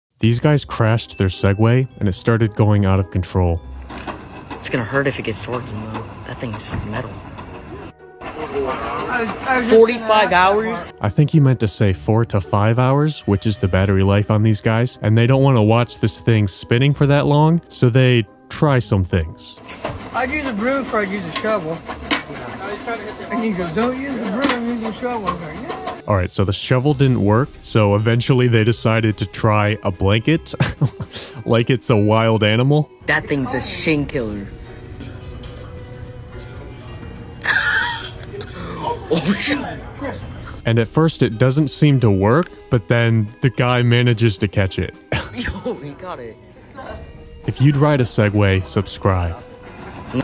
Their Segway started spinning 😬
Their Segway crashed and it started spinning out of control.